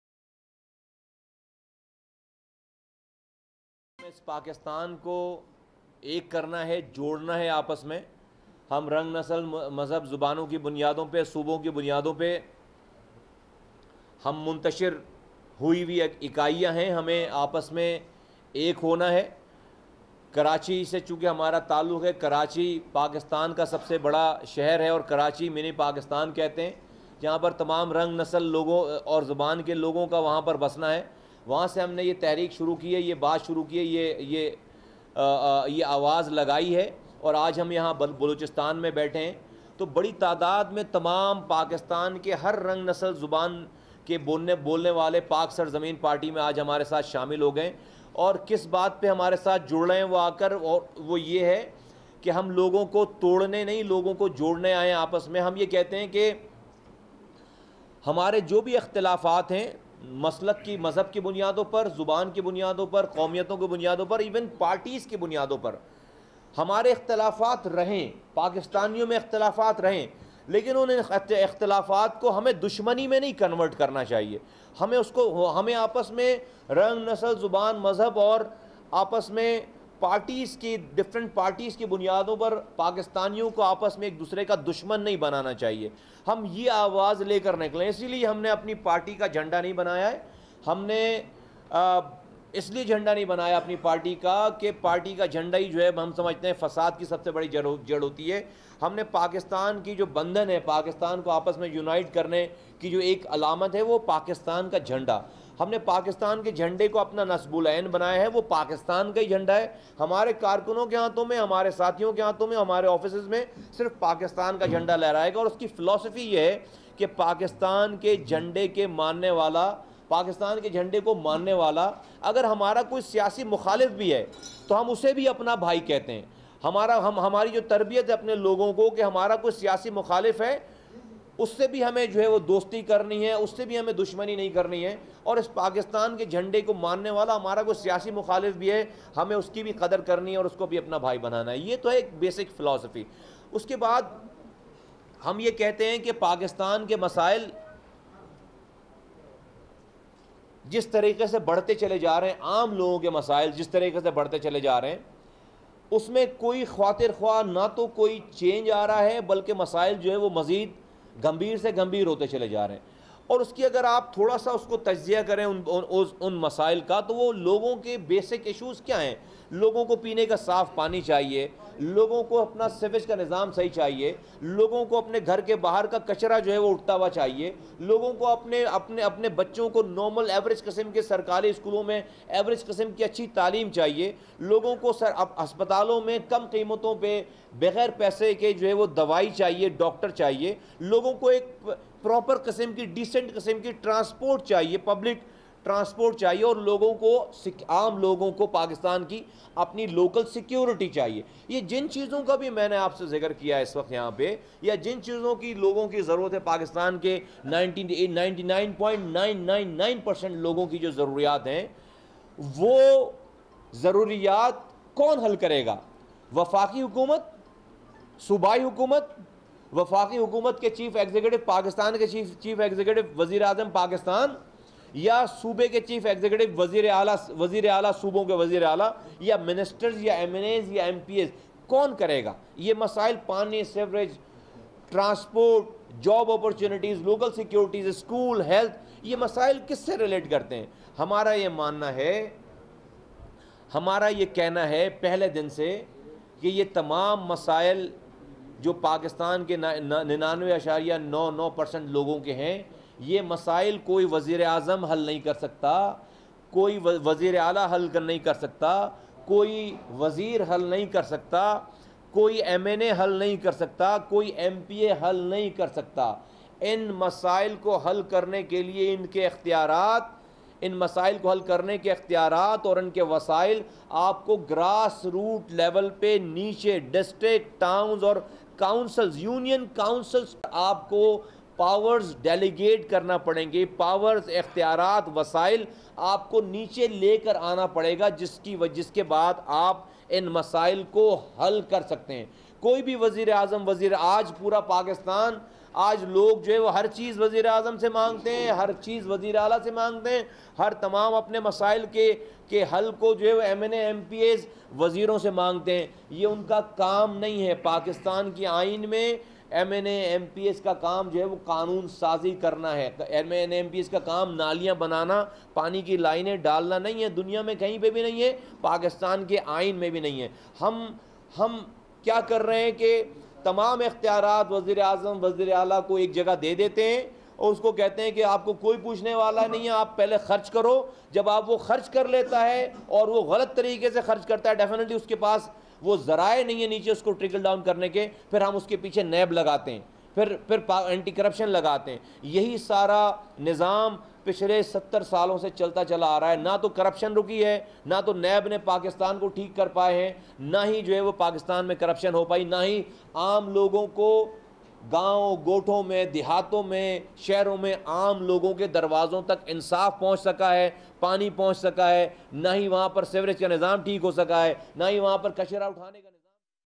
24 Live: Mustafa Kamal press conference in Quetta
24 Live: Mustafa Kamal press conference in Quetta